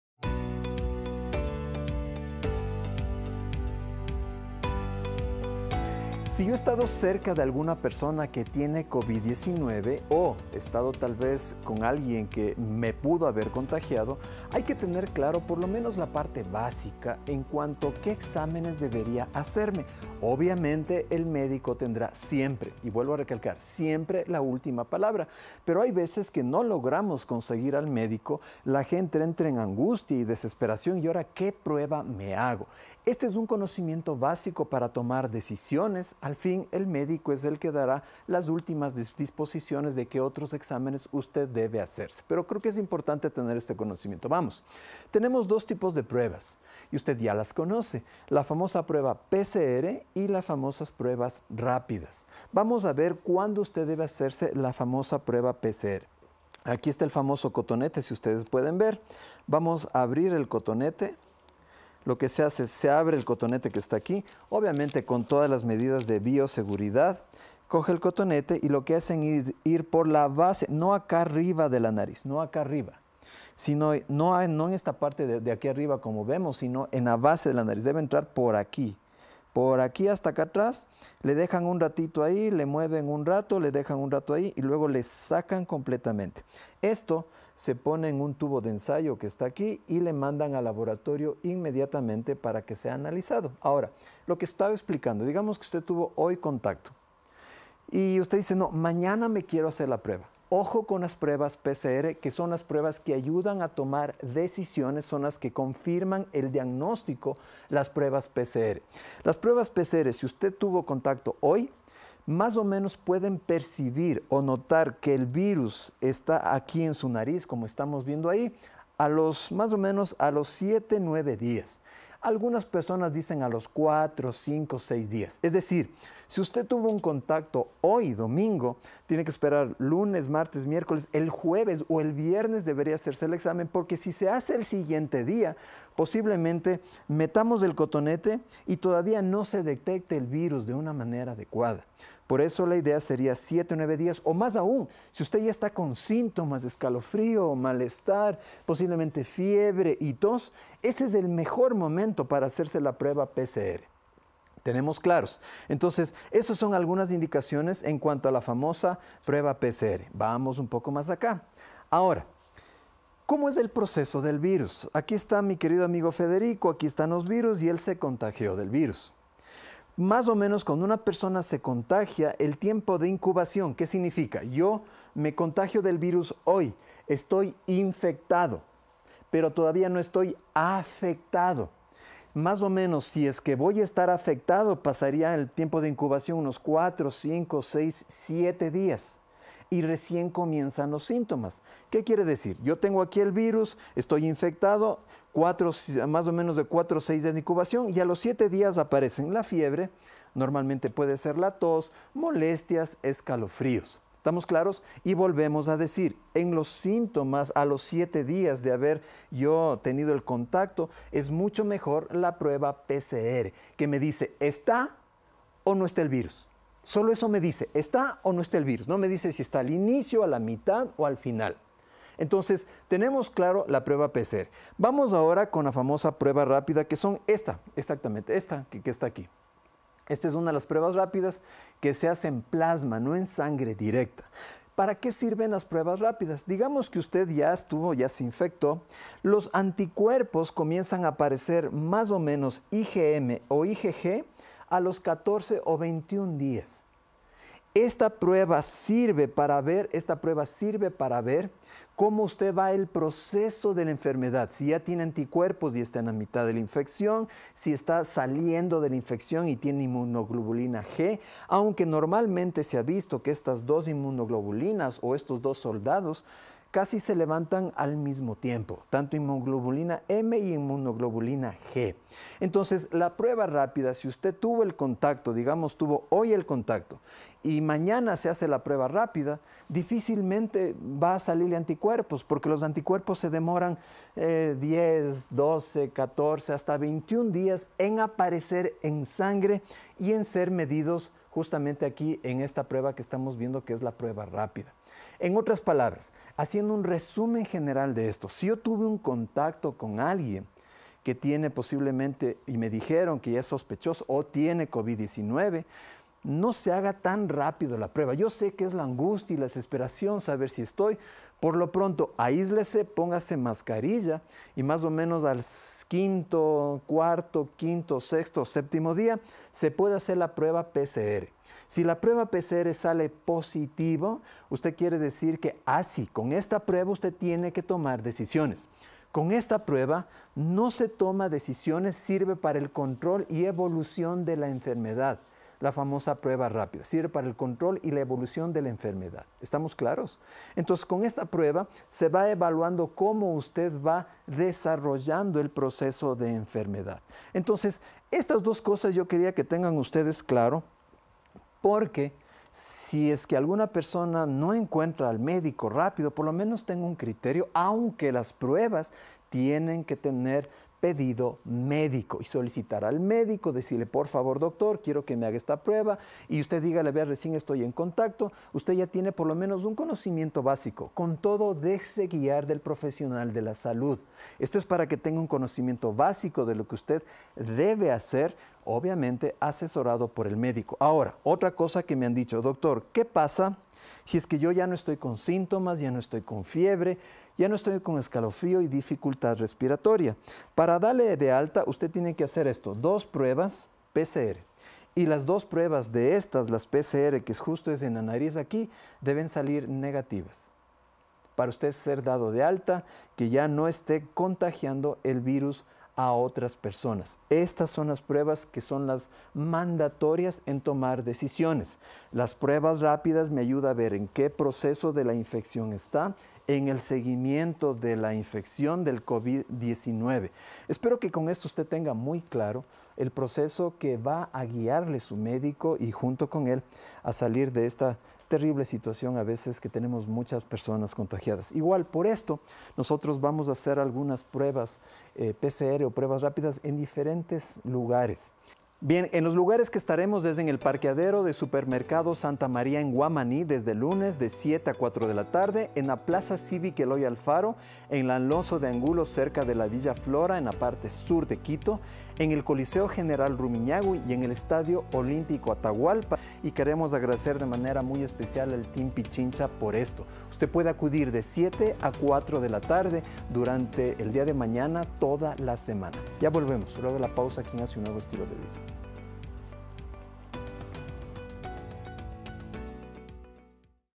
PRUEBAS COVID 19 ¿CUÁL ME HAGO?
Conoce las diferentes pruebas que existen en el mercado actual para confirmar si una persona es contagiado por este virus llamado Covid 19, además cuando es necesario hacerlo.